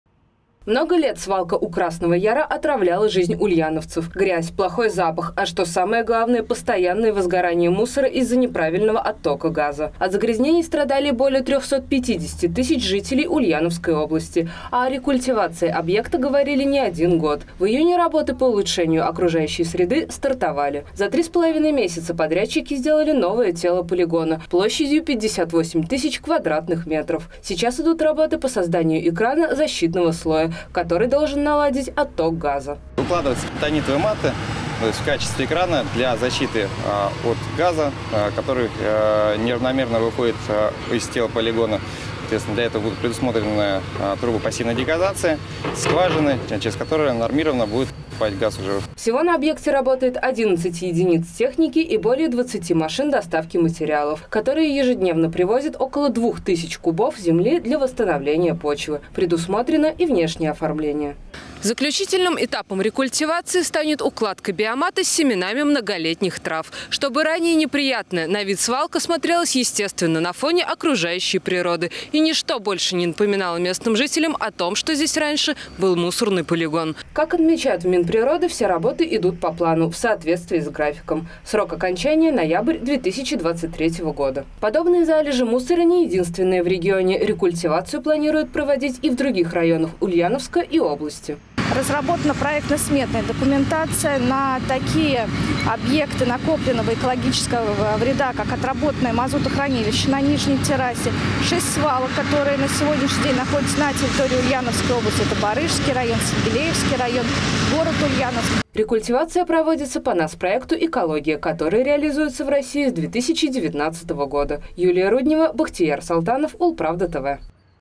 Создают защитный слой. У Красного Яра рекультивируют свалку
Мусорный полигон в Чердаклинском районе Ульяновской области сходит на нет. Десятки лет свалка доставляла местным неудобства. В этом году началась долгожданная рекультивация на месте свалки. Экватор работ пройден и вскоре на месте скопления мусора будет красивая возвышенность.